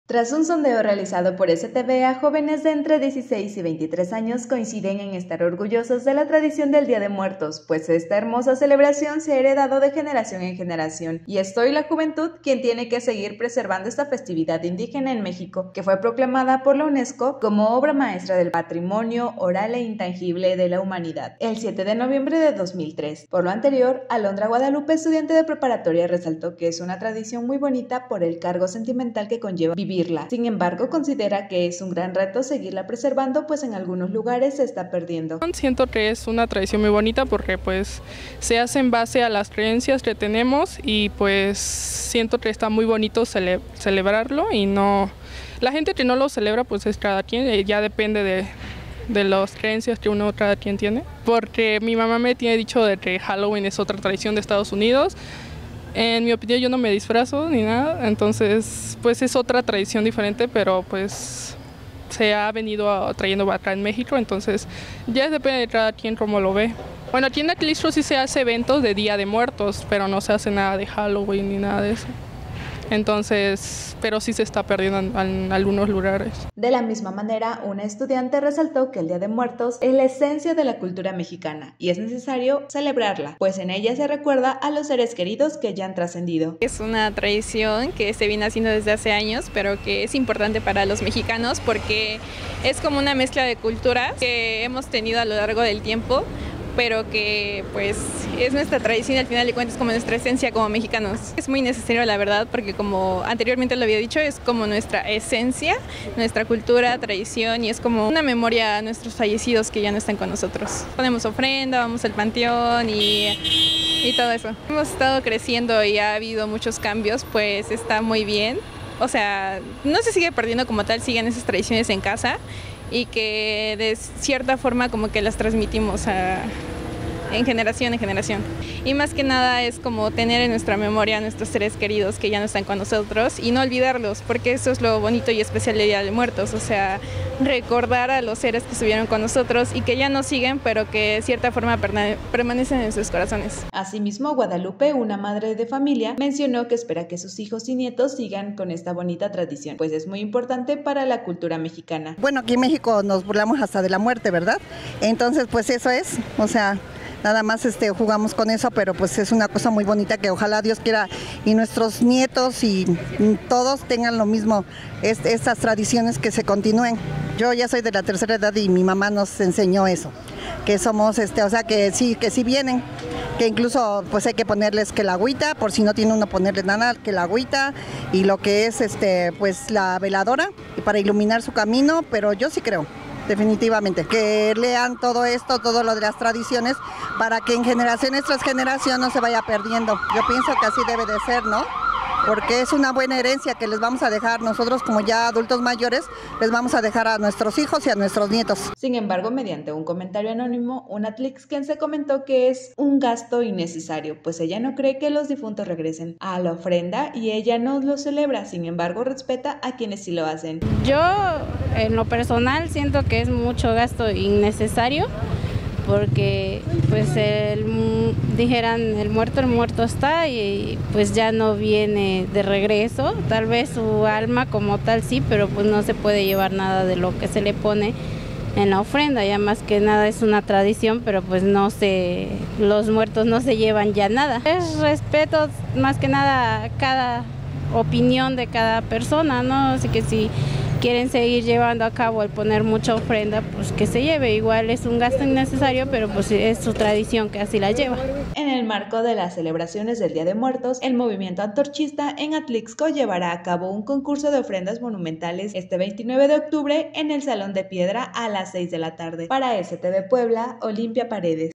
Entérate STV🟣Jóvenes coinciden en estar orgullosos de la tradición del Día de Muertos
Entérate STV🟣 Tras un sondeo realizado por STV a jóvenes de entre 16 a 23 años, coinciden en estar orgullosos de la tradición del Día de Muertos, pues esta hermosa celebración se ha heredado de generación en generación y es hoy, la juventud, quien tiene que seguir preservando esta festividad indígena en México que fue proclamada por la Unesco como “Obra Maestra del Patrimonio Oral e Intangible de la Humanidad” el 7 de noviembre de 2003.